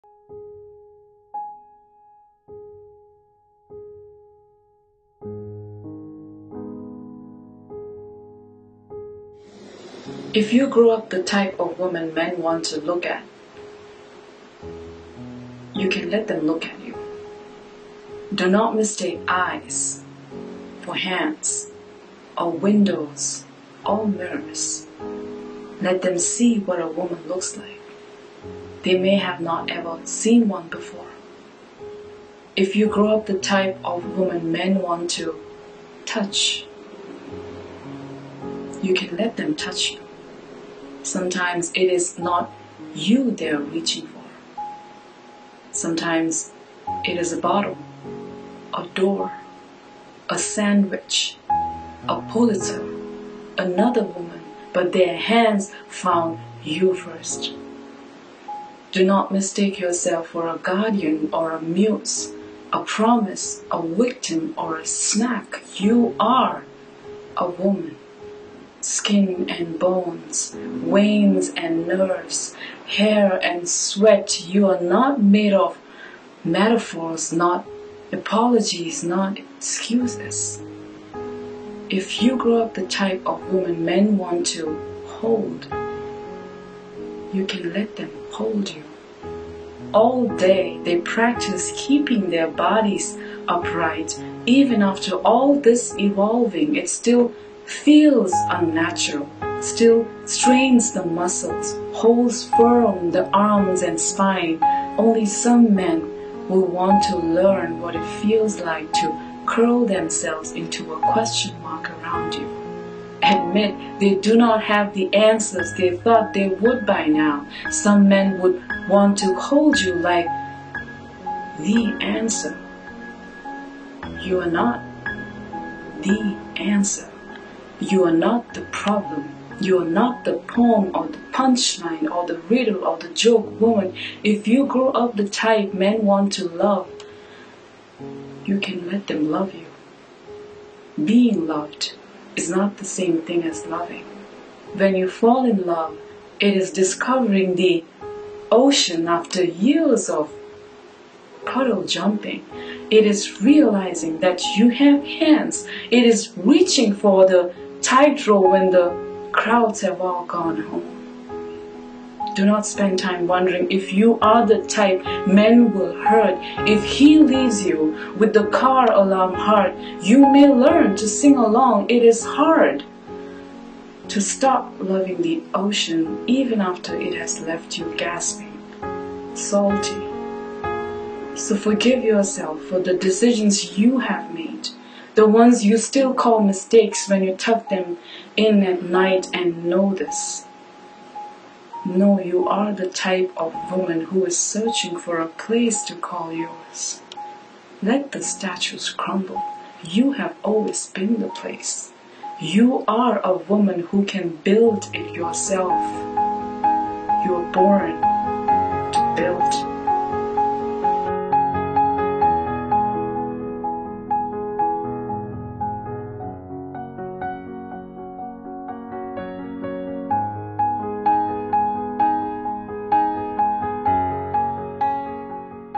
The Type By Sarah Kay
Here's a short video of me performing on one of my favorite poems, The Type, by Sarah Kay. This poem, I hope, will resonate with a lot of women who are disillusioned by the society with its patriarchy, misogyny and double standards. I hope it will remind every women to not beat themselves up for the choices they make in their lives. Because the society is often brutal with women and their choices, so we cannot afford to be a complicit and let the same thing happen to ourselves. I hope this poem will remind every women that their very existence, is the testimony of their strength and resilience.
Camera: Selima Smith-Dell
Concept & Editing: Tsering Dolma Daktsa